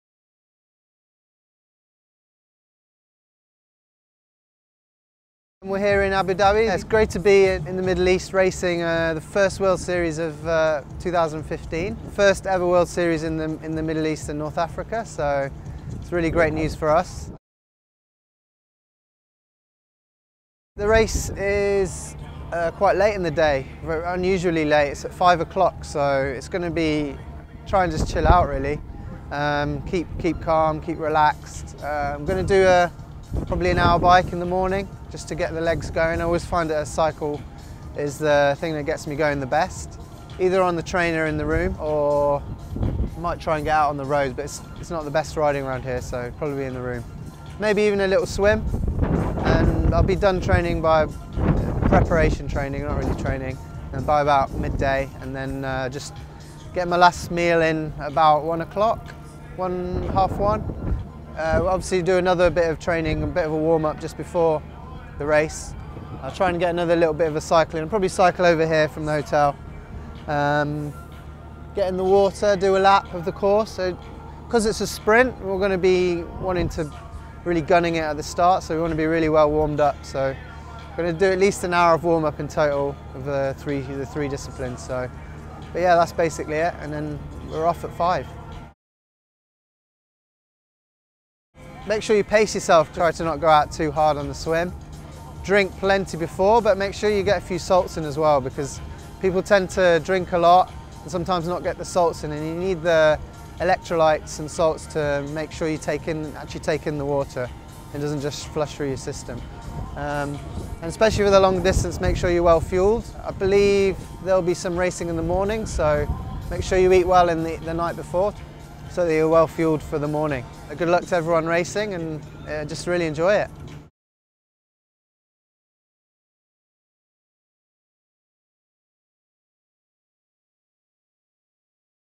Lawrence Fanous Pre-Race - WTS AbuDhabi 2015
March 2015: Alameda o.n. Tri Team pro athlete, Lawrence Fanous, talks about his upcoming race, ITU World Triathlon Abu Dhabi 2015; preparation and race tips.